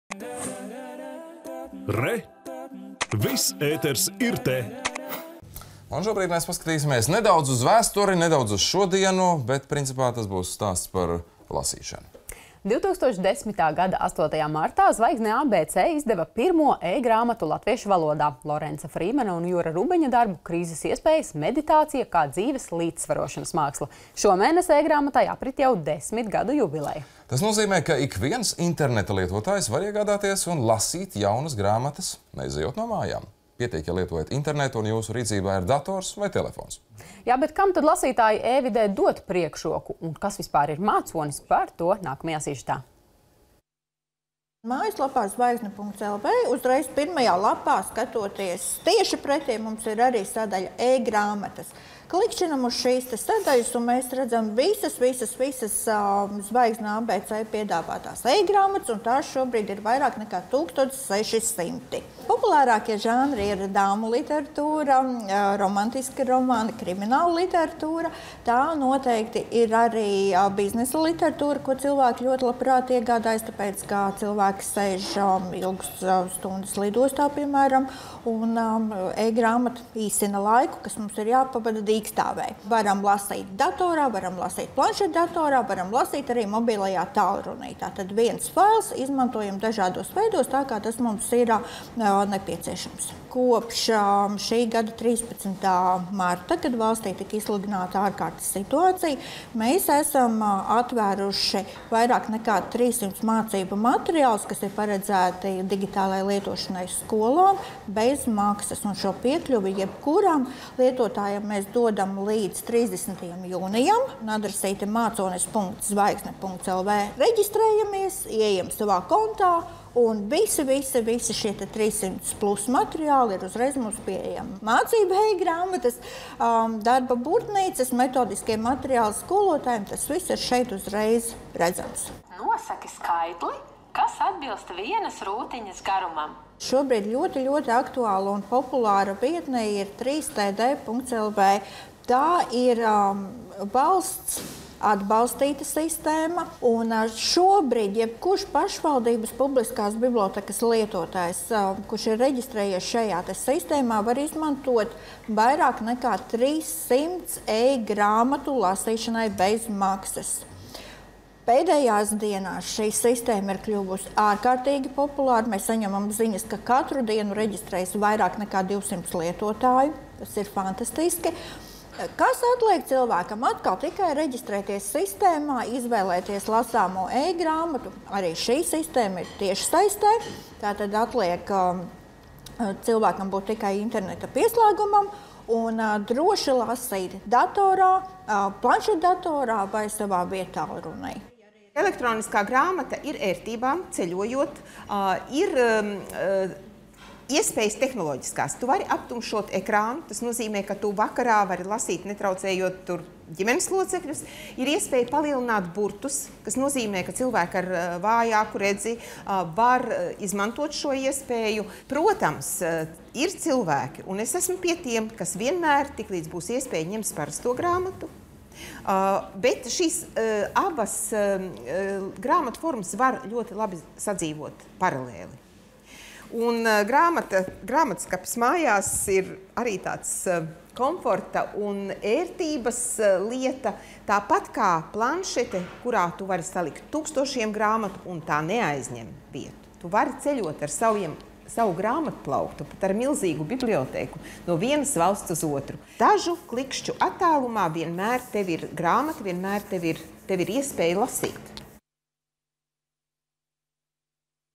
E - grāmatai 10
2010. gada 8. martā Zvaigzne ABC izdeva pirmo e-grāmatu latviešu valodā .– Lorensa Frīmena un Jura Rubeņa darbu “Krīzes iespējas. Meditācija kā dzīves līdzsvarošanas māksla”.Šogad E grāmatai 10 gadu jubileja.
Tas nozīmē, ka ikviens interneta lietotājs var iegādāties un lasīt jaunas grāmatas, neizejot no mājas. Pietiek, ja lietojat internetu un jūsu rīcībā ir dators vai telefons.
Kam lasītāji e vidē dod priekšroku un kas ir MĀCONIS...par to nākamajā sižetā.